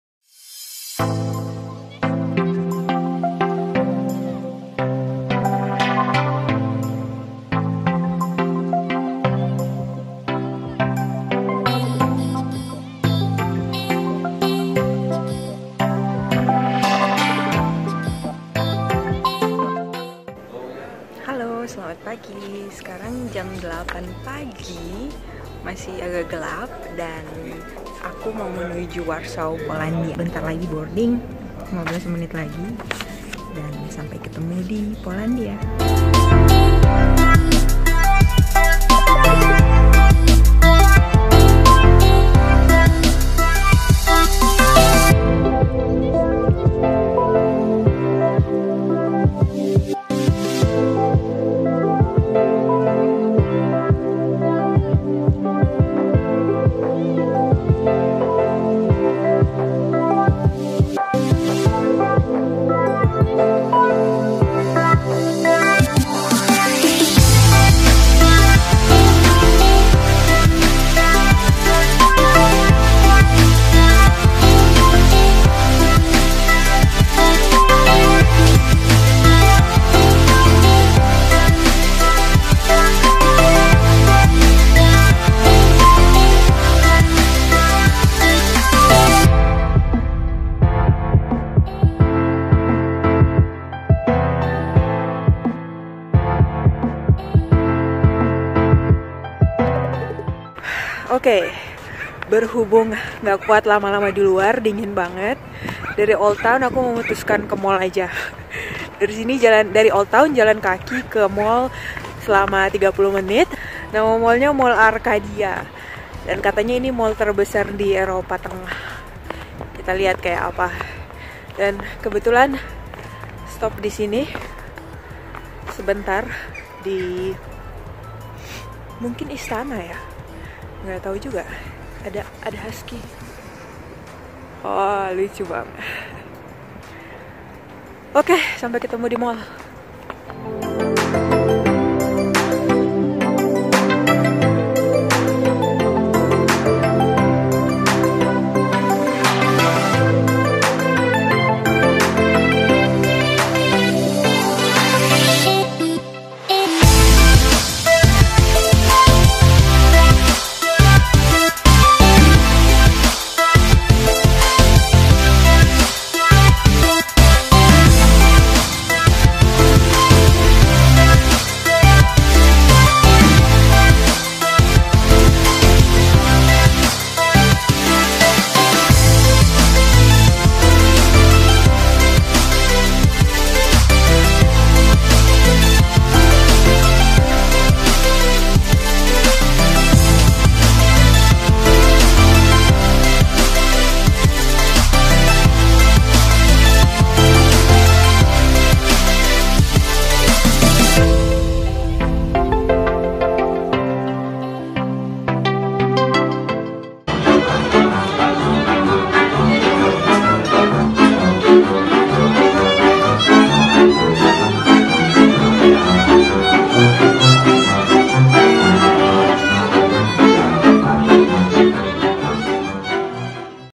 Trip singkat ke Warsaw Polandia
Hallo, nama aku Zhira.
Di video pertama ini aku bakal jalan-jalan ke kota Warsaw di Polandia. Selamat tonton! :)